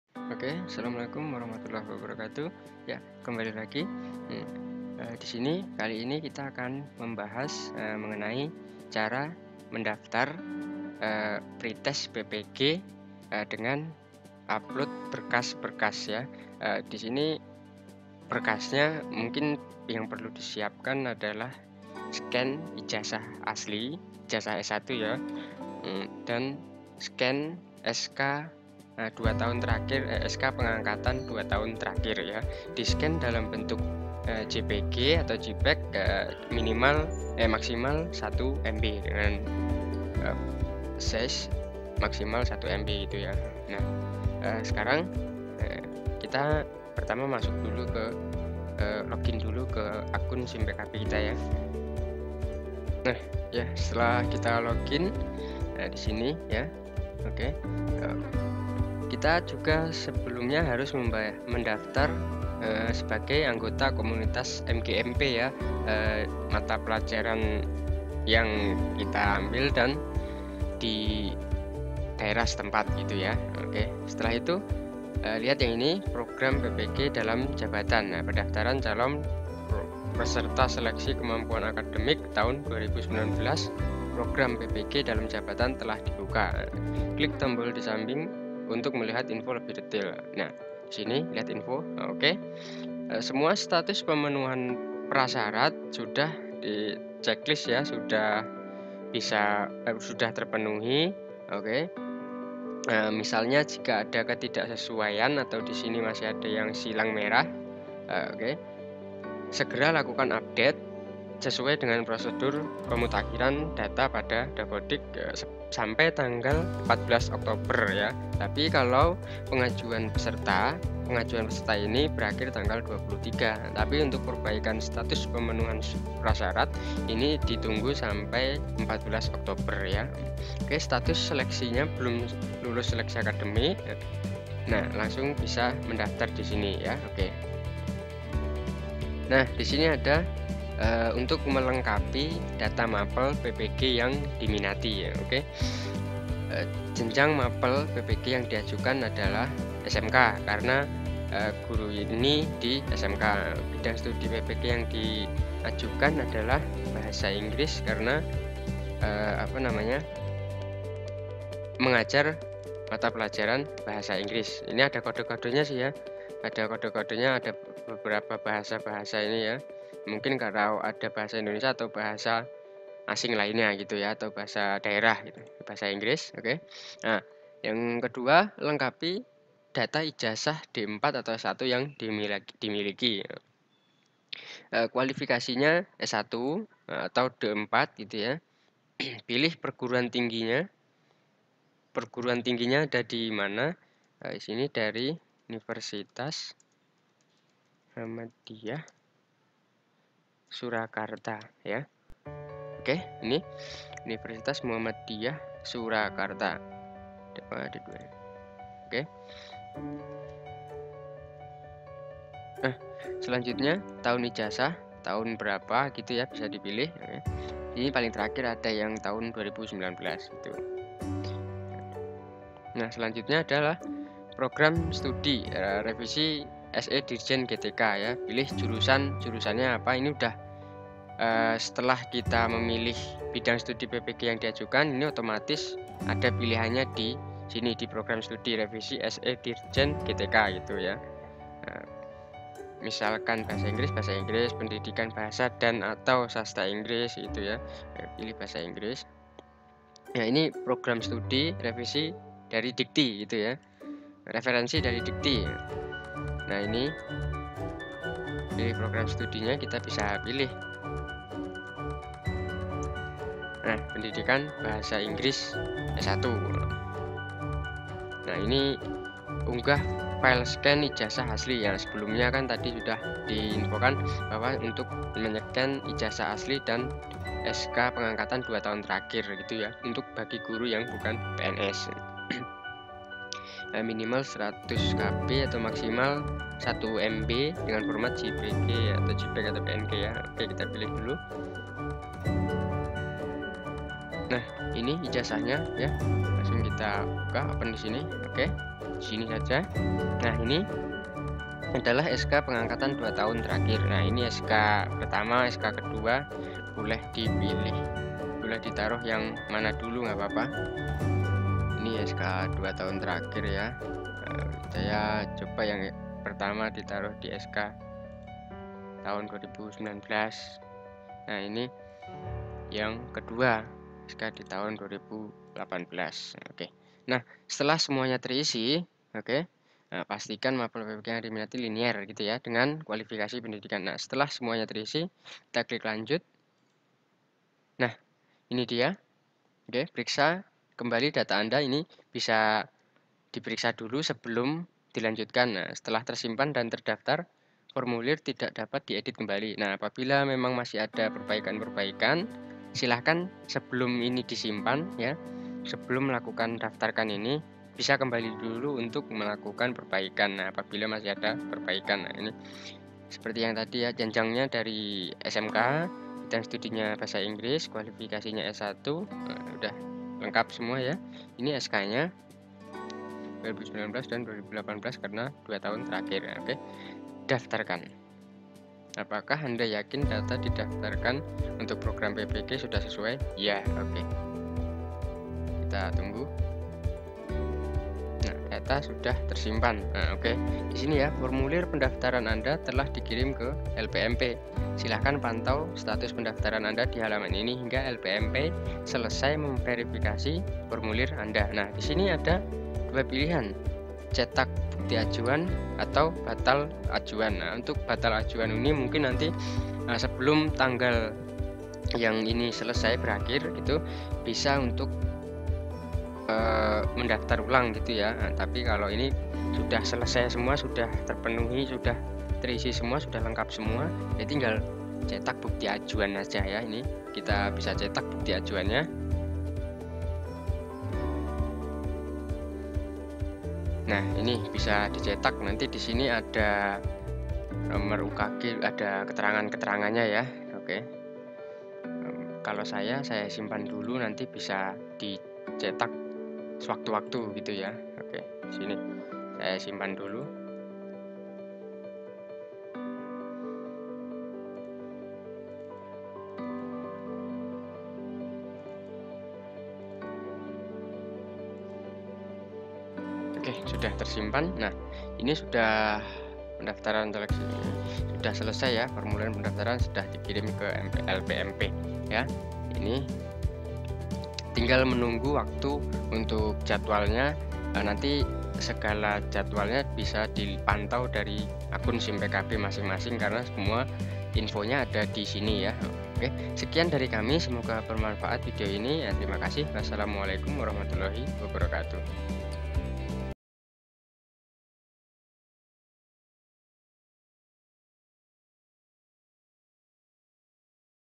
CARA MENDAFTAR PPG (PROG. PENDIDIKAN PROFESI GURU)
Program PPG Dalam Jabatan
Pendaftaran Calon Peserta Seleksi Kemampuan Akademik tahun 2019 Prog. PPG Dalam Jabatan telah dibuka.